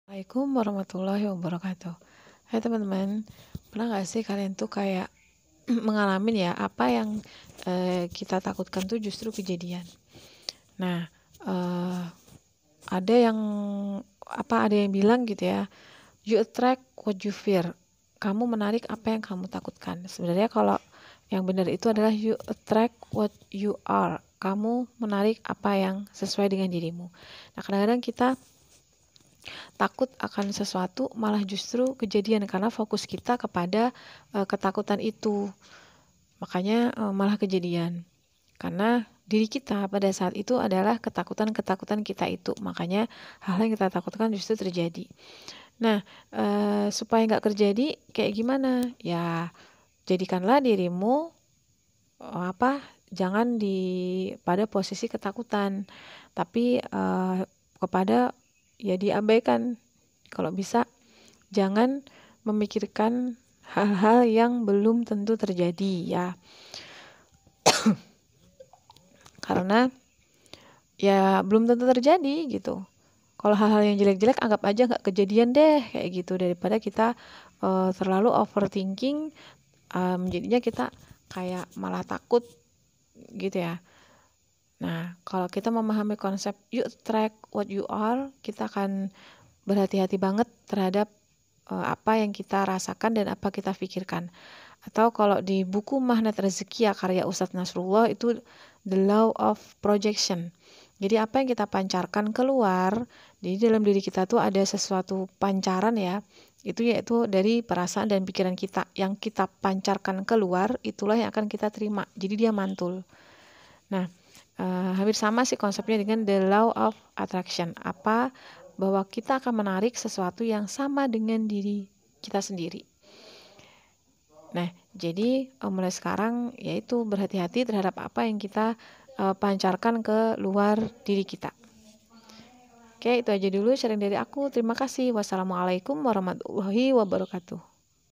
KEKUATAN PIKIRAN DAN PERASAAN | HATI-HATI DENGAN PIKIRAN DAN PERASAAN | LAW OF ATTRACTION
apa saja yang kita pancarkan dari pikiran dan perasaan atas kondisi atau peristiwa yang terjadi akan menjadi nasib kita berikutnya. Mereka yang memahami ini akan lebih mudah fokus kepada apa yang diinginkan, bukan apa yang ditakutkan. Simak video pendek ini ya  :)
Bagi teman-teman yang butuh referensi buku bacaan untuk memulai perubahan diri, bisa klik tautan ini ya :